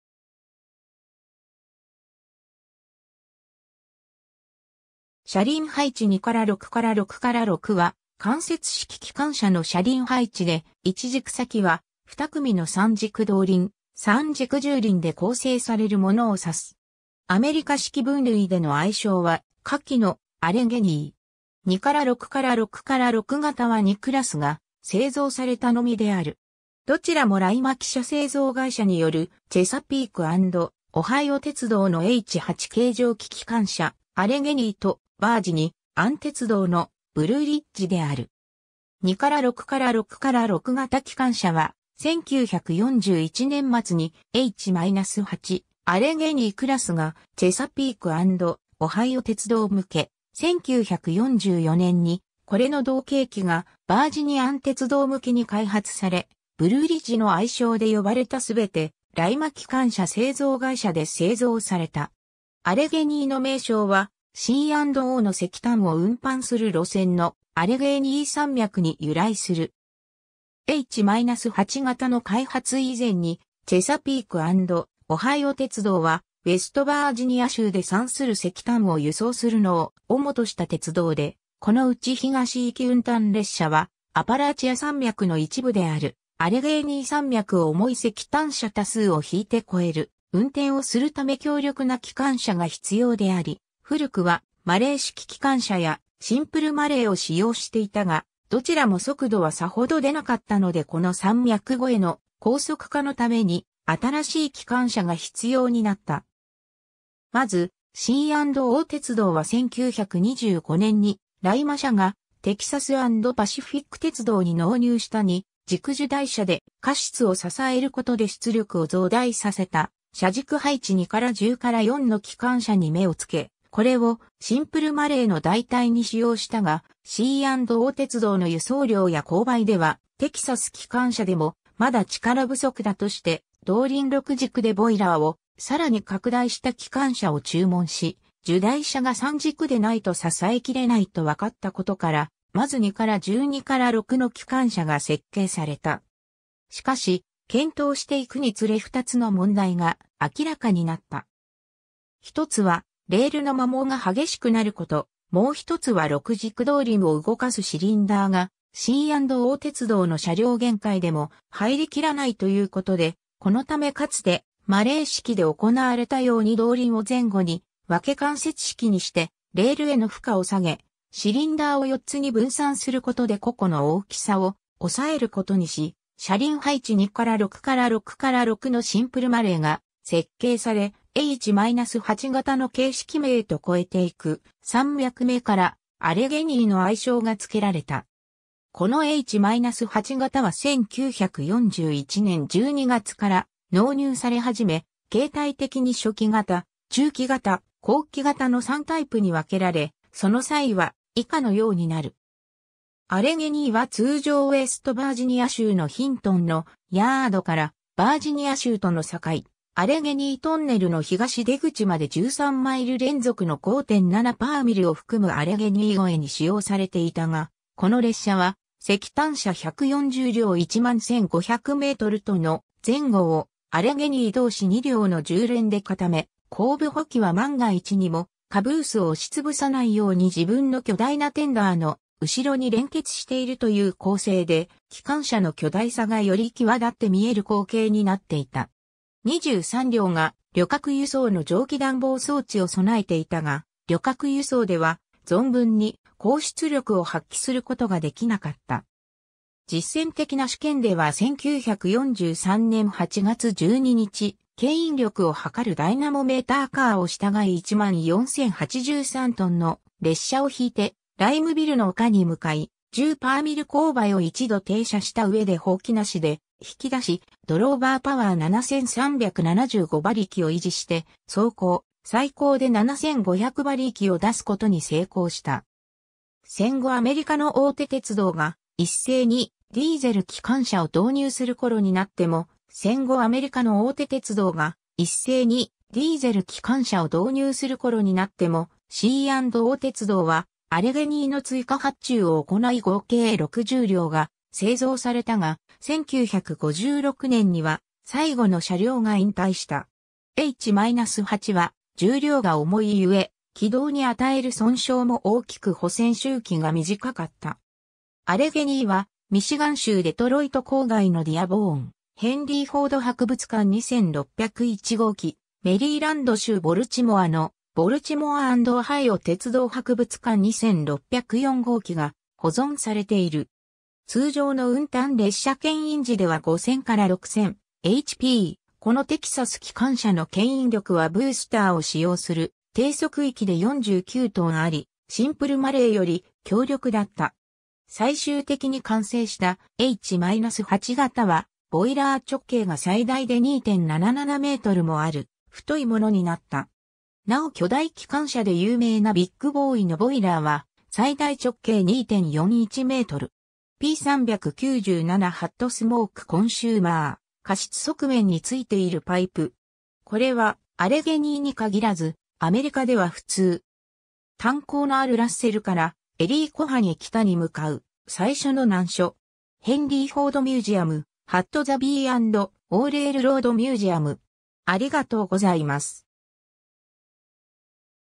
車輪配置 2-6-6-6
車輪配置2-6-6-6の機関車
車輪配置 2-6-6-6 (しゃりんはいち2-6-6-6、ホワイト式分類)は関節式機関車の車輪配置で、1軸先輪・2組の3軸動輪・3軸従輪で構成されるものをさす。
アメリカ式分類での愛称は下記の「アレゲニー」（または「アレゲニィ」）。
 2-6-6-6型は2クラスが製造されたのみである。
どちらもライマ汽車製造会社によるチェサピーク・アンド・オハイオ鉄道のH8形蒸気機関車「アレゲニー」とバージニアン鉄道の「ブルーリッジ」である。
 2-6-6-6型機関車は、1941年末にH-8「アレゲニー」クラスがチェサピーク・アンド・オハイオ鉄道(C&O)向け、1944年にこれの同型機がヴァージニアン鉄道向けに開発され「ブルーリッジ」の愛称で呼ばれた（ヴァージニアン鉄道での正式名は「AG型」）すべてライマ機関車製造会社で製造された。
アレゲニーの名称はC&Oの石炭を運搬する路線のアレゲーニー山脈に由来する。
 H-8型の開発以前に、チェサピーク・アンド・オハイオ鉄道（以下、C&O鉄道）はウェストヴァージニア州で産する石炭を輸送するのを主とした鉄道で、このうち東行き運炭列車はアパラチア山脈の一部であるアレゲーニー山脈を重い石炭車多数を牽いて越える運転をするため強力な機関車が必要であり、古くはマレー式機関車（本来の複式のもの）や、シンプルマレー（車輪配置2-8-8-2）を使用していたが、どちらも速度はさほど出なかったのでこの山脈越えの高速化のために新しい機関車が必要になった。
 まず、C&O鉄道は1925年にライマ社がテキサス＆パシフィック鉄道に納入した2軸従台車で火室を支えることで出力を増大させた車軸配置2-10-4（テキサス）の機関車 に目をつけ、これをシンプルマレーの代替に使用したが、C&O鉄道の輸送量や勾配ではテキサス機関車でもまだ力不足だとして動輪6軸でボイラーをさらに拡大した機関車を注文し、従台車が3軸でないと支え切れないと分かったことからまず2-12-6の機関車が設計された。
 しかし、検討していくにつれ2つの問題が明らかになった。
1つは（固定軸距が長いので）レールの摩耗が激しくなること、もう1つは6軸動輪を動かすシリンダーがC&O鉄道の車両限界でも入りきらないという事で、このためかつてマレー式で行われたように動輪を前後に分け関節式にしてレールへの負荷を下げ、シリンダーを4つに分散する（複式にはしない）ことで個々の大きさを抑えることにし、車輪配置2-6-6-6のシンプルマレーが設計され、H-8型の形式名と越えていく山脈名から「アレゲニー」の愛称が付けられた。
 このH-8型は1941年12月から納入されはじめ、形態的に初期型・中期型・後期型の3タイプに分けられ、その差異は以下のようになる。
 アレゲニー（H-8）は通常ウェスト・ヴァージニア州のヒントン（Hinton）のヤードからヴァージニア州との境、アレゲニートンネルの東出口まで13マイル連続の5.7‰を含むアレゲニー越えに使用されていたが、この列車は石炭車140両・11500米トン（メートルトンでは10430t）の前後をアレゲニー同士2両の重連で固め、後部補機は万が一にもカブース（車掌車）を押しつぶさないように自分の巨大なテンダーの後ろに連結しているという構成で、機関車の巨大さがより際立って見える光景になっていた。
 23両が旅客輸送の蒸気暖房装置を備えていたが旅客輸送では存分に高出力を発揮する事ができなかった。
 実戦的な試験では1943年8月12日、牽引力を測るダイナモメーターカーを従え14083tの列車を引いてライムビルの丘 に向かい、10‰勾配を一度停車したうえで補機なしで引き出しドローバーパワー7375馬力を維持して走行、最高で7500馬力（時速74㎞時）を出すことに成功した。
 戦後アメリカの大手鉄道が一斉にディーゼル機関車を導入するころになっても、戦後アメリカの大手鉄道が一斉にディーゼル機関車を導入するころになっても、C&O鉄道はアレゲニーの追加発注を行い（上記表の「後期型」）合計60両が製造されたが、1956年には最後の車両が引退した。
 H-8は重量が重い故、軌道に与える損傷も大きく保線周期が短かった。
 アレゲニーはミシガン州デトロイト郊外のディアボーン「ヘンリーフォード博物館 」に1601号機、メリーランド州ボルチモアの「ボルチモア・アンド・オハイオ鉄道博物館 」に1604号機が保存されている。
 ^ 通常の運炭列車牽引時...